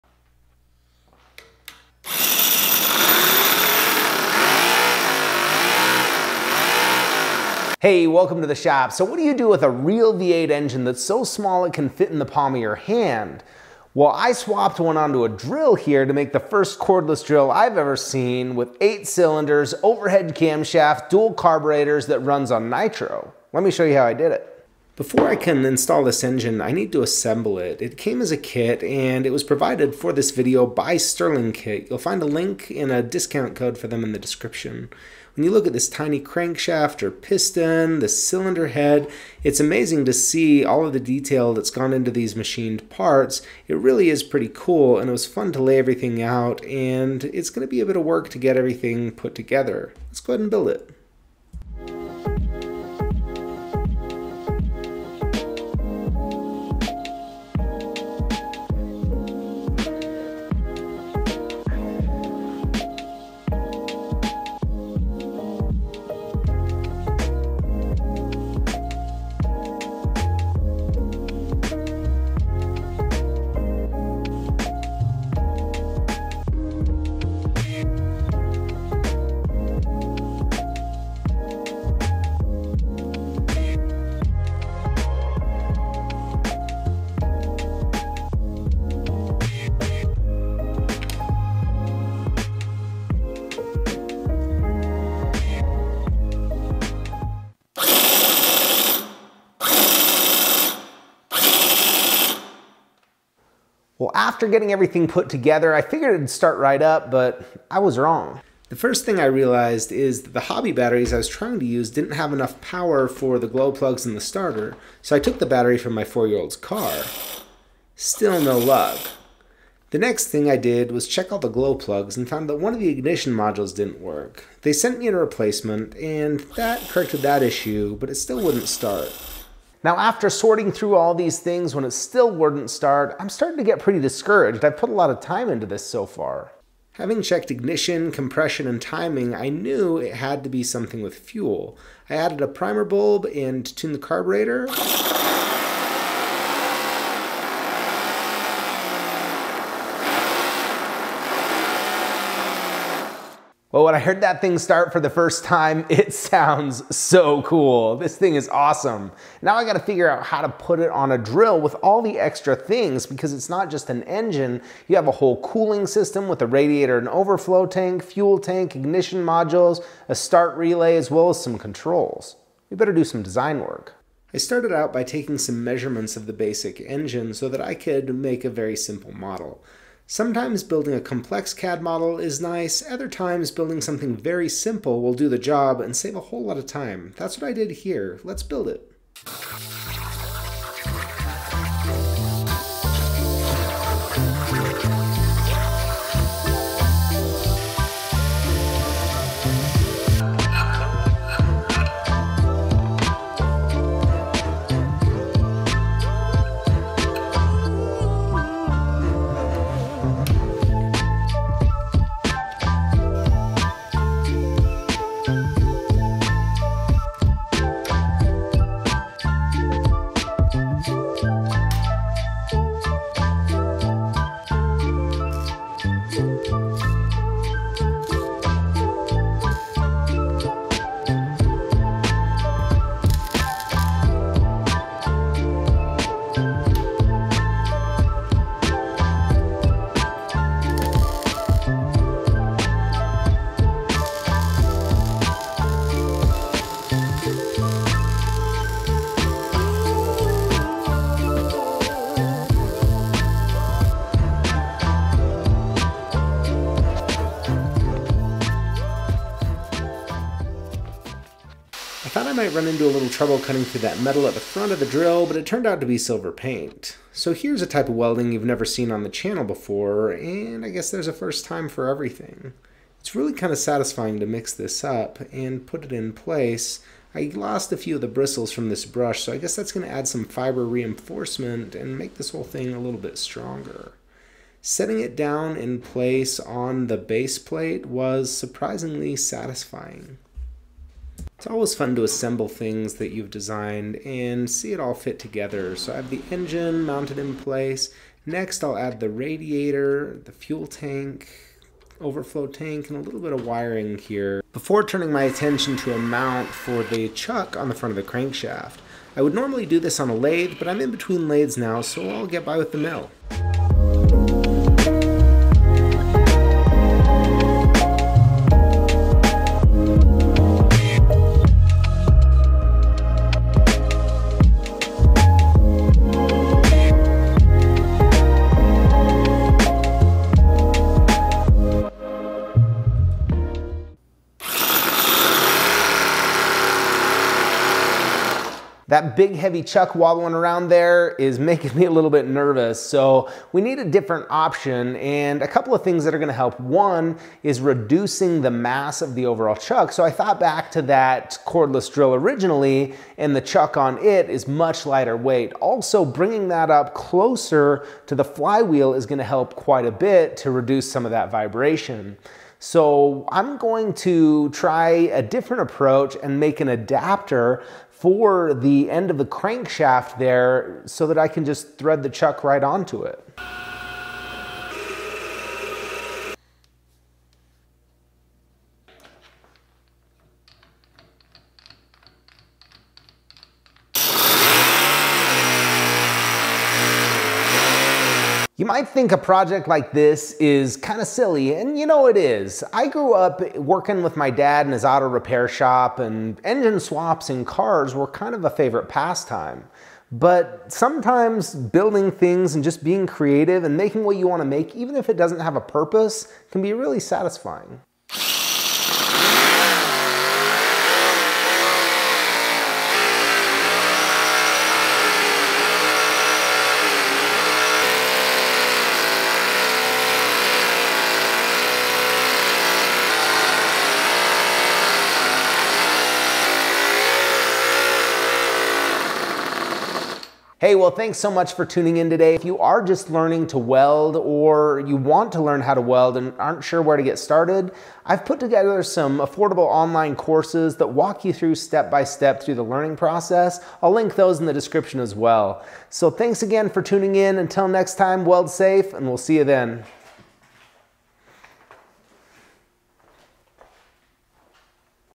I put a NITRO POWERED V8 ENGINE on my DRILL!
What isn't better with a V8?